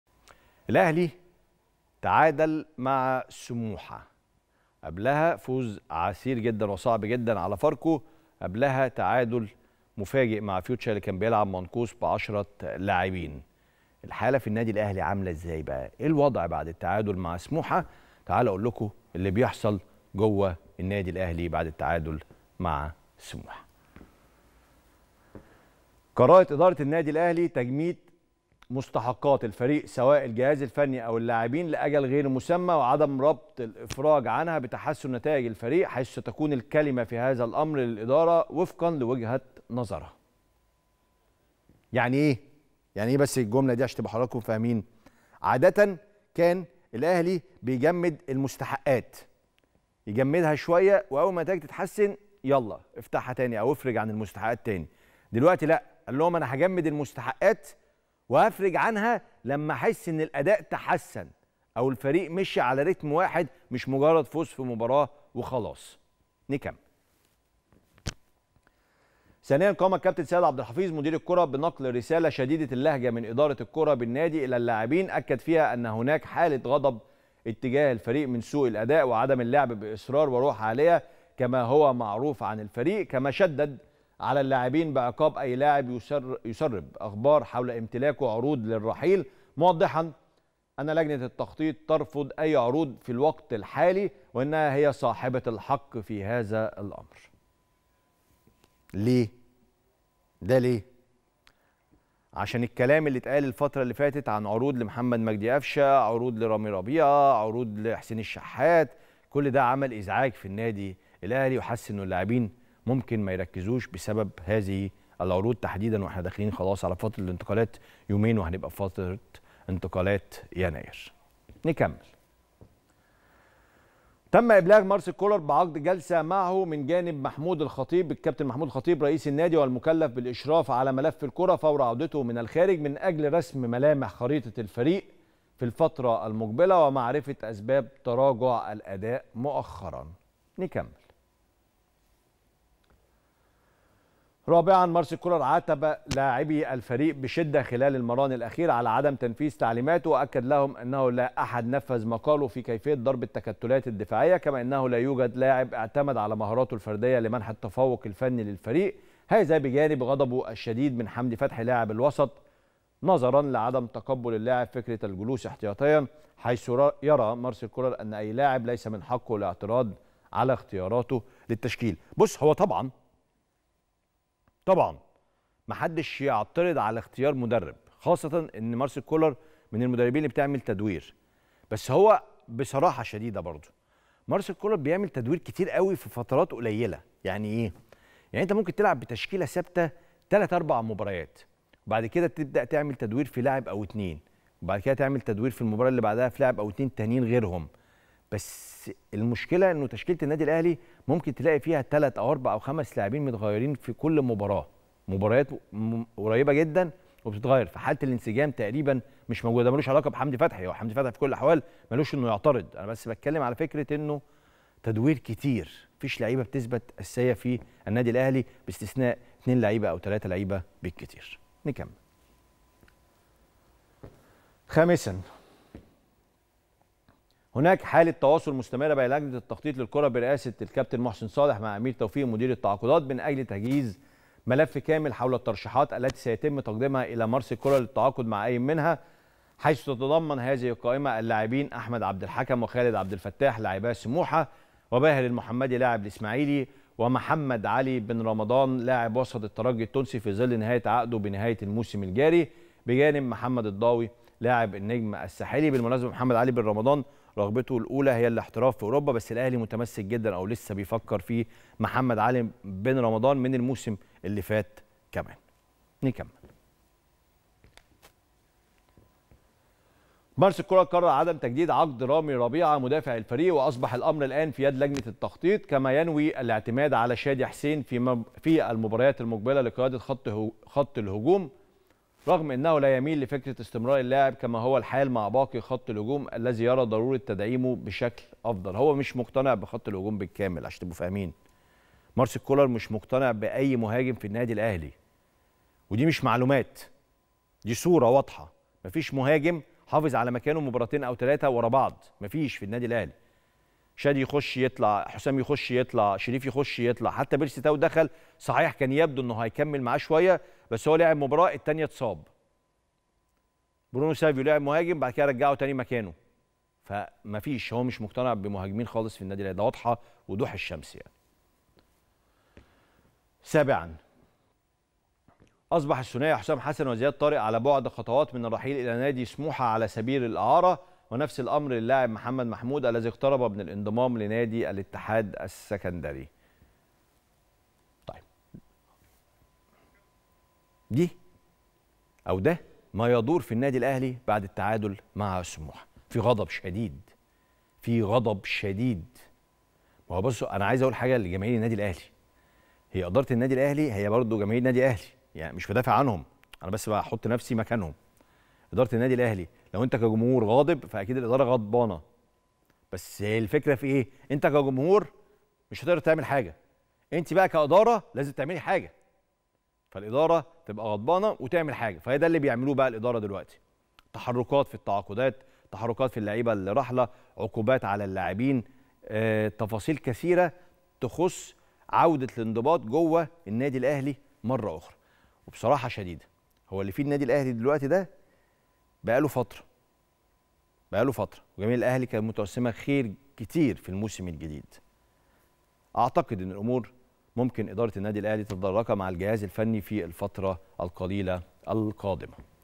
قرارات صارمة وعقوبات وانتقاد كولر للاعبين ويصب غضبه على حمدي فتحي💣💣.. ماذا يحدث داخل الأهلي 🔥🔥
قرارات صارمة وعقوبات وانتقاد كولر للاعبين ويصب غضبه على حمدي فتحي💣💣.. ماذا يحدث داخل الأهلي بعد التعادل أمام سموحة 🔥🔥